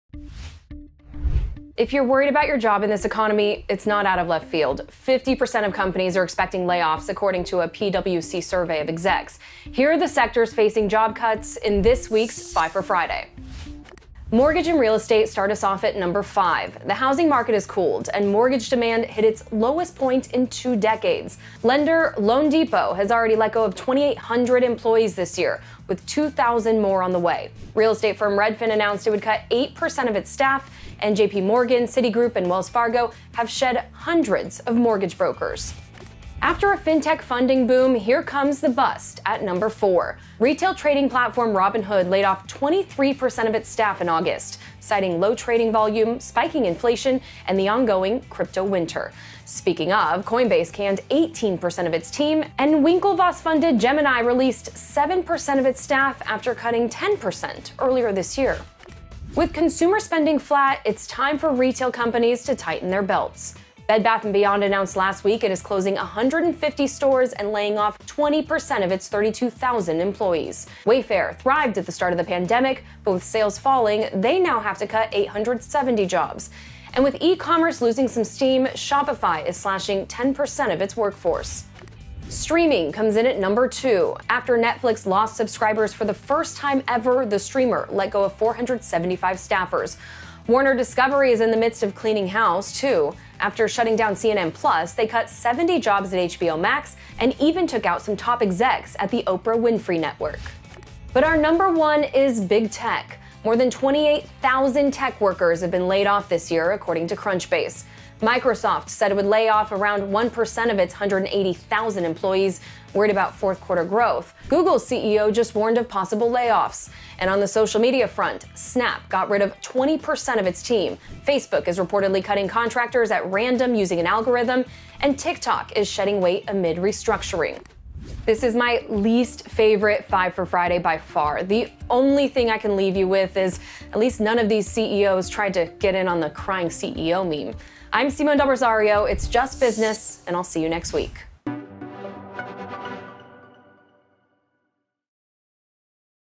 Five For Friday | Five sectors facing layoffs amid economic uncertainty and high inflation
Concerns about the state of the economy come with worries about job security. In fact, half of companies are expecting to cut staff according to a PwC survey of more than 700 executives. The same survey found many executives are also planning hiring freezes and even rescinding job offers. But many companies have already trimmed their workforce. Here are the sectors facing layoffs in this week’s Five For Friday.